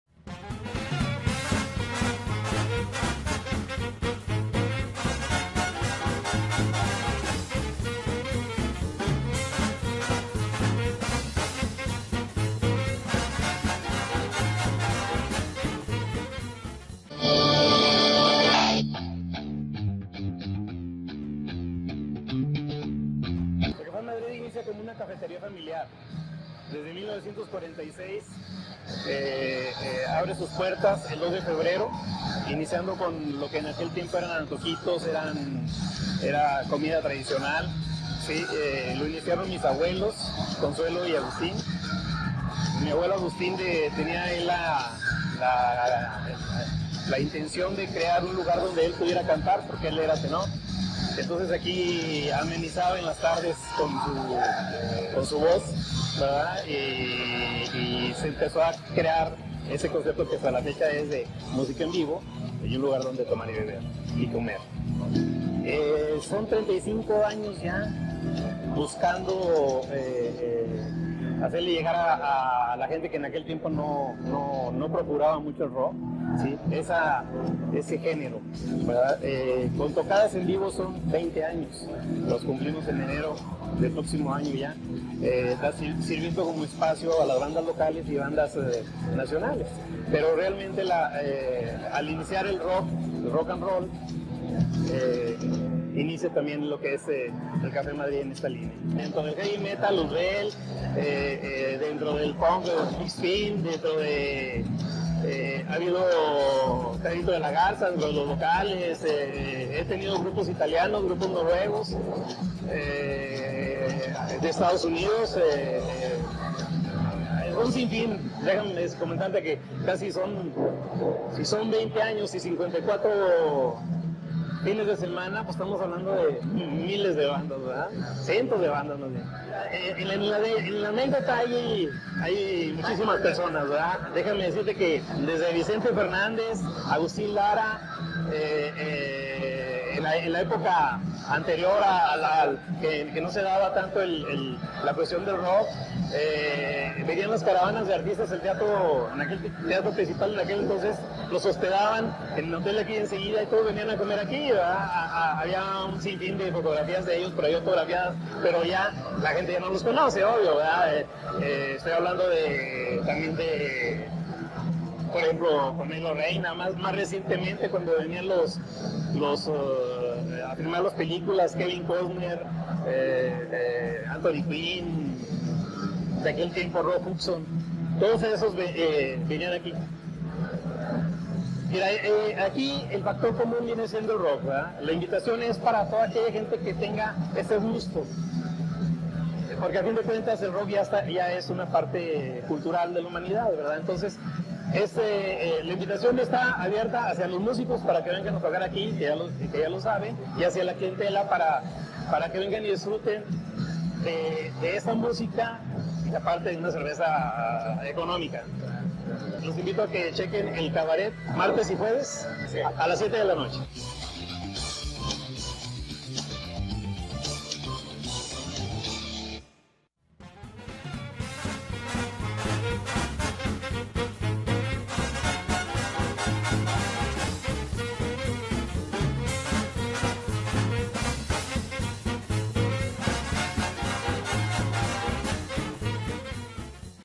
DDPM: Café Madrid Rock Place
Martes 17 de septiembre de 2019
En esta ocasión en el segmento De Durango Para el Mundo, visitamos el "Café Madrid-Rock Place" en donde nos platica su dueño Gerardo Pescador Alcalde, ¿quiénes son?, ¿desde cuándo se creó el lugar?, ¿quiénes lo han visitado?, el servicio que brindan, y  que artistas conocidos han visitado el lugar, como por ejemplo Vicente Fernández, Cornelio Reina, Kevin Costner, Anthony Quinn, entre otros y nos expresa que es un lugar para los que tienen el gusto por el Rock, acompáñanos a verlo…

🔘 Diego Rentería Hernández Twitter      🐦: @DRenteriah
🔘 Heber García Hernández Twitter         🐦: @heber_GH
🔘 Leonardo Álvarez Cervantes Twitter   🐦: @leon_alvarez